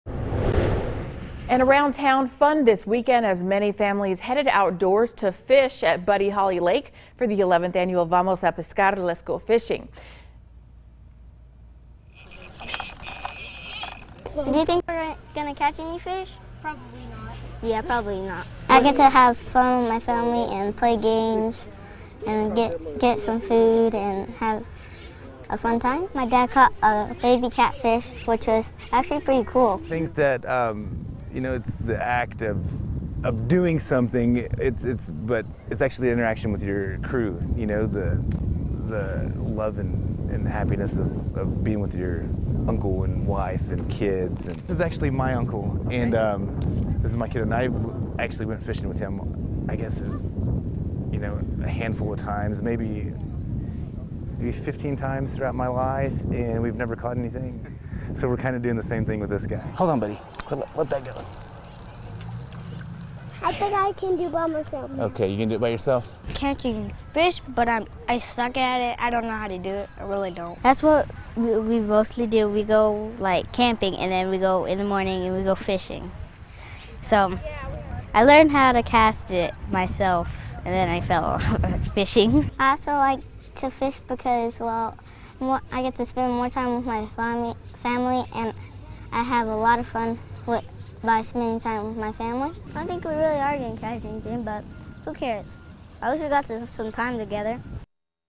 11th Annual “Vamos a Pescar, Let’s Go Fishing” (KLBK)
The annual event is hosted by Los Hermanos Familia at Buddy Holly Lake.